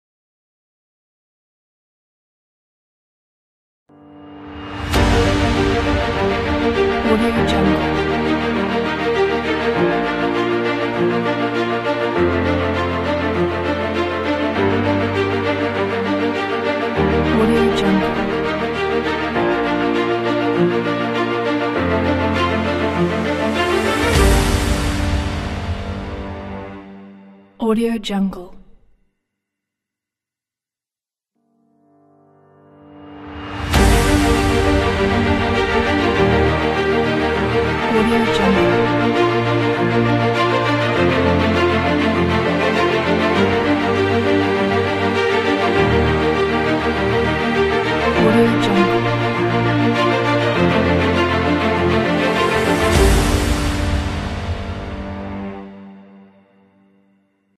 Gold Time
Duration 00:27, 30fps, 1920×1080 resolution .After Effects CS6, CC, CC 2014, CC 2015 and above .Adapted to any logo or text .Very easy to change logo and text .fast render time .No plug-ins required .uses font Trajan Pro .3D renders Pre-rendered